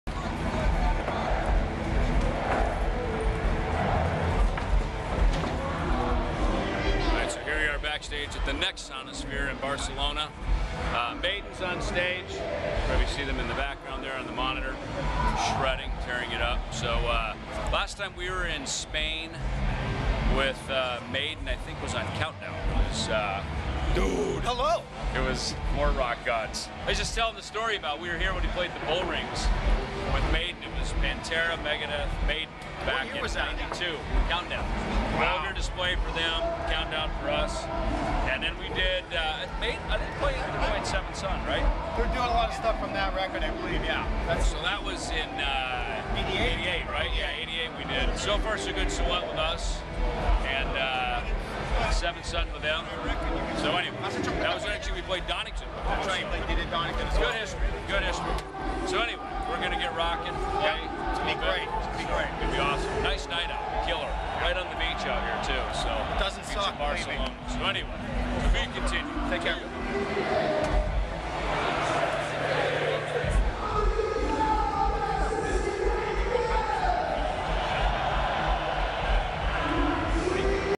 David Ellefson & Shawn Drover of MEGADETH Barcelona Sonishphere 2013
Megadeth backstage at Barcelona, Spain Sonisphere 2013 with Iron Maiden, Anthrax, Newsted, Ghost and more.